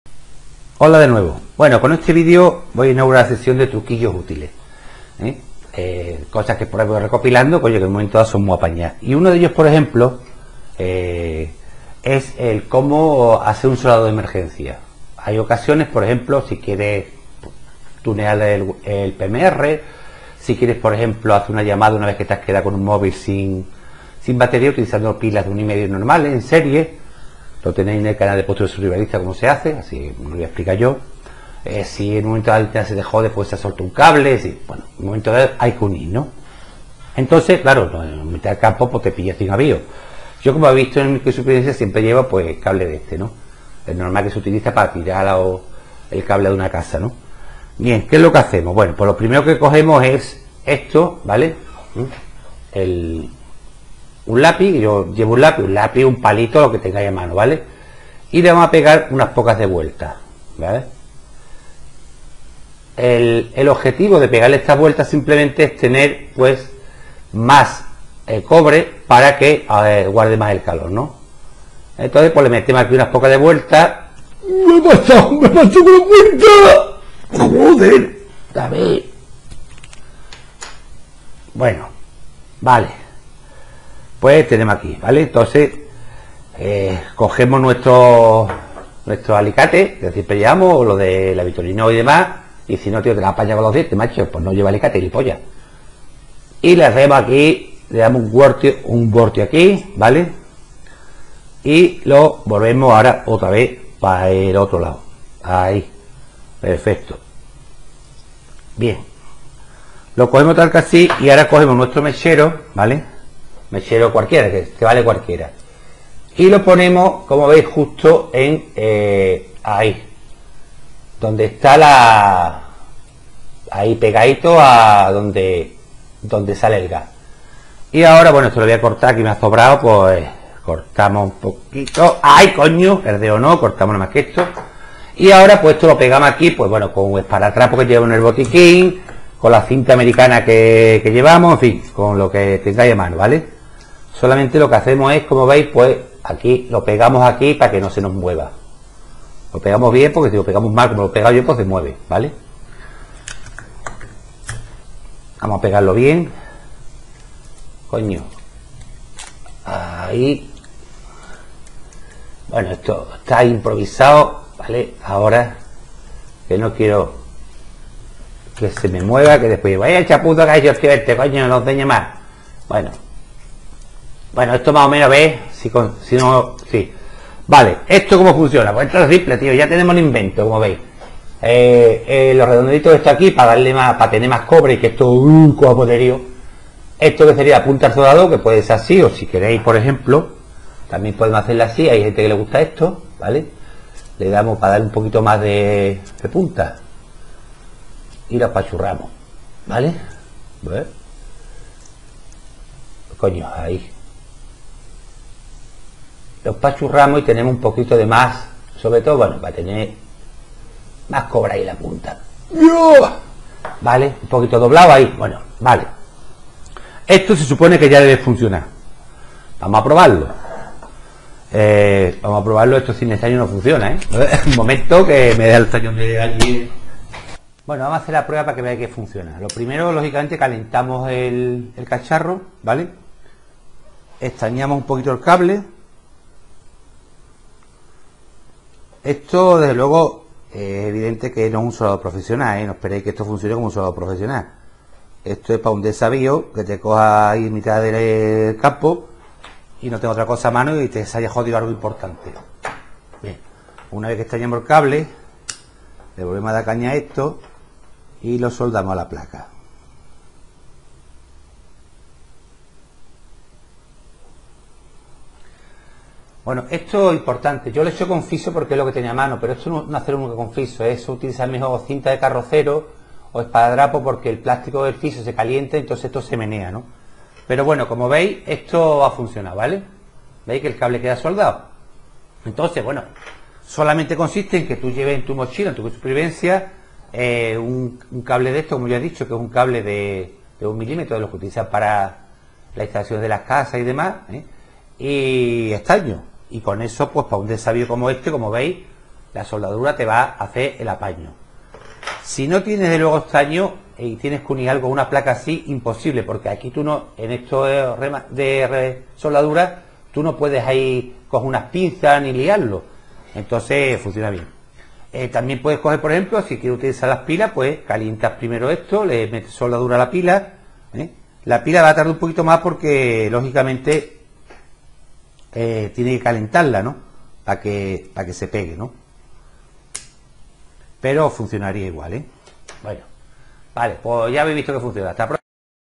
Soldador de emergencia con un mechero
Como hacer un soldador con un mechero
Este es el video de Postureo Survivalero que os recomiendo que veais, como hacer una llamada, con el móvil, sin batería, utilizando las pilas normales de 1.5, (R6 o AAA)